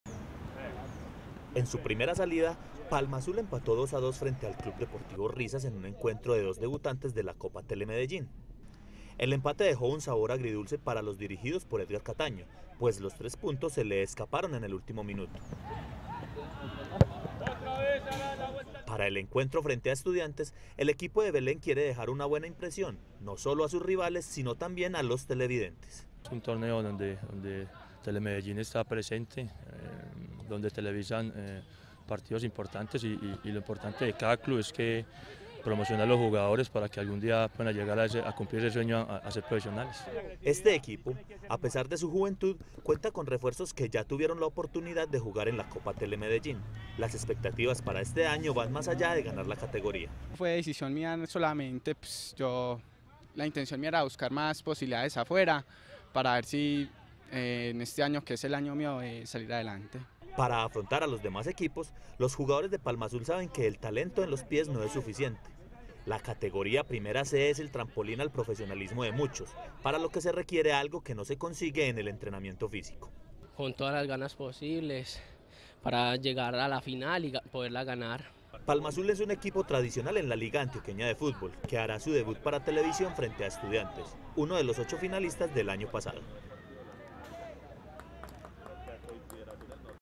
Palmazul debutará en la segunda edición de la Copa Telemedellín
En su Primera salida, Palmazul empató 2-2 frente al club deportivo Risas en un encuentro de dos debutantes de la Copa Telemedellín. El empate dejó un sabor agridulce para los dirigidos por Edgar Cataño, pues los tres puntos se le escaparon en el último minuto.